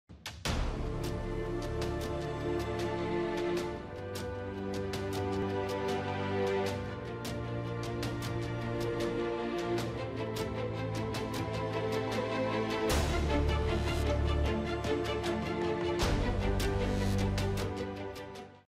4Dfx by 4Dviews - Time Effects
Slow motion, frozen moment, reserve, fast forward: with 4Dfx you can now edit and modify the playback of your volumetric video sequences.